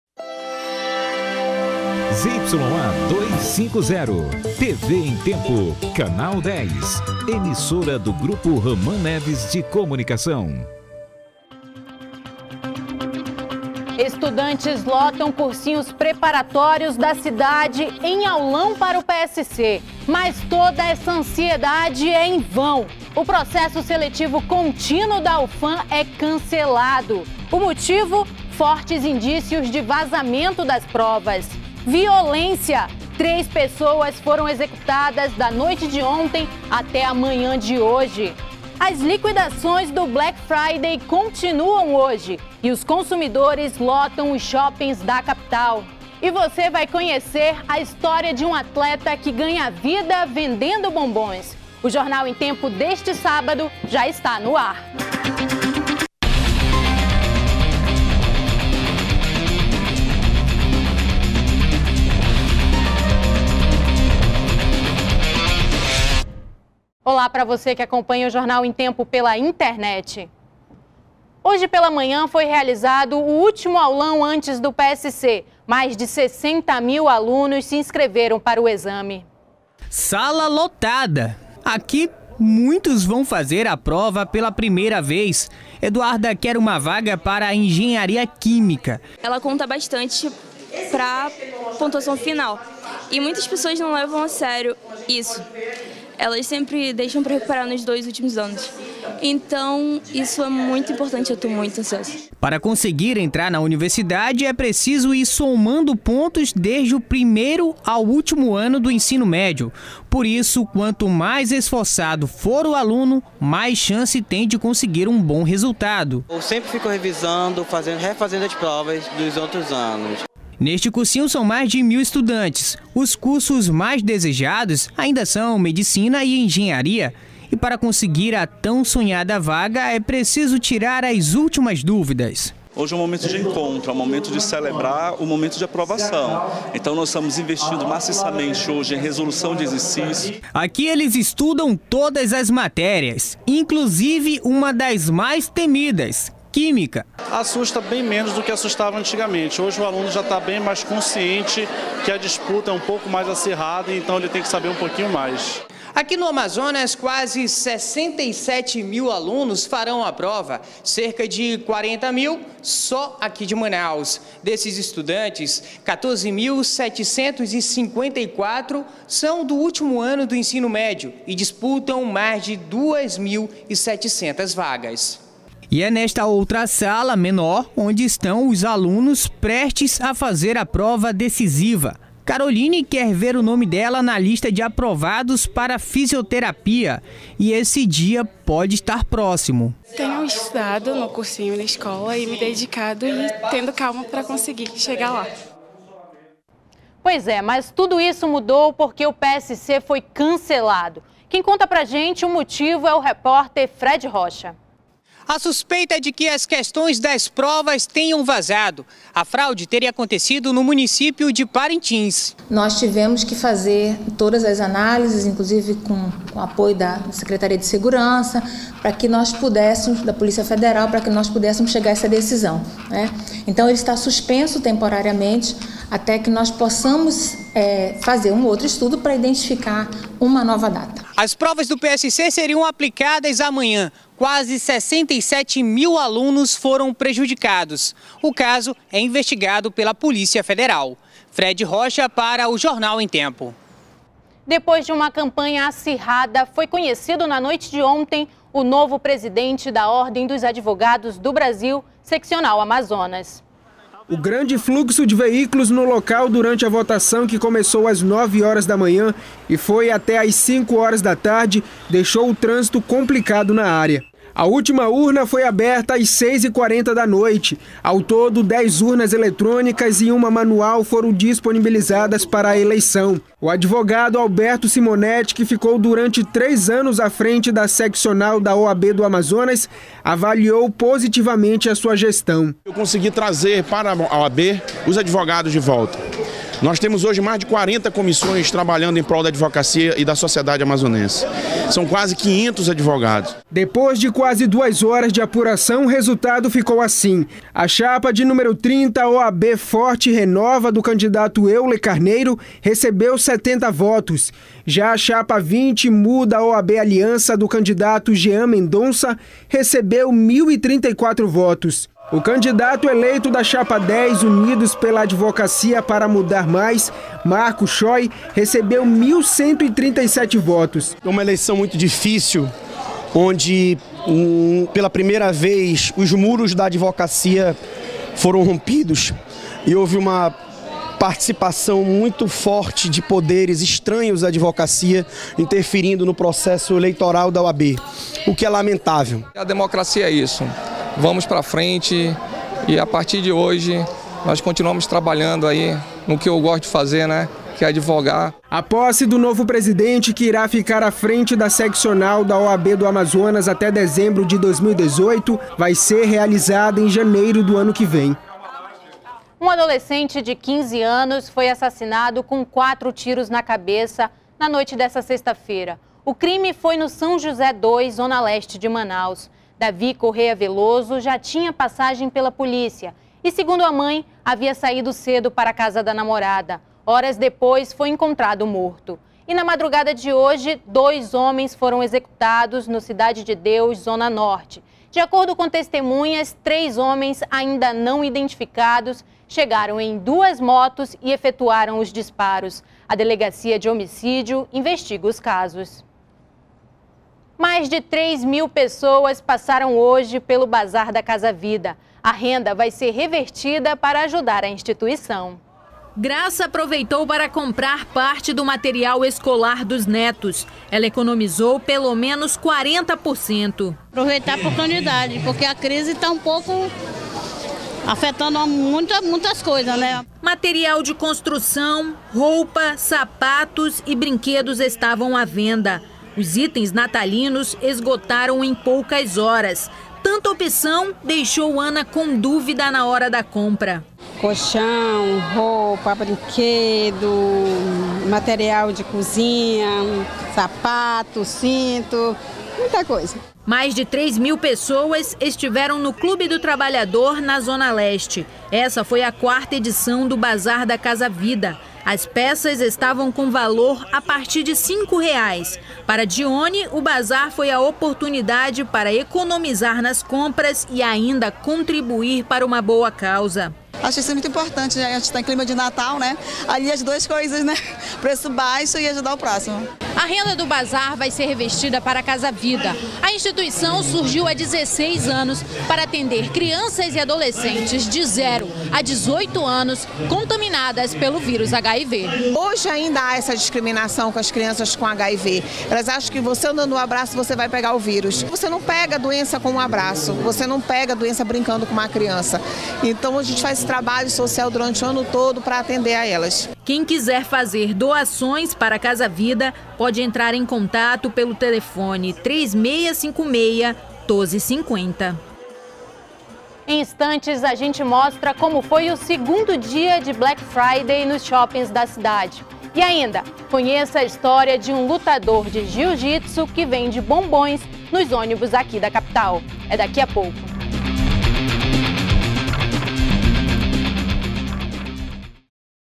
PSC da UFAM é cancelado por fortes indícios de vazamento das provas
01º BLOCO JORNAL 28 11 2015 - PSC da UFAM é cancelado por fortes indícios de vazamento das provas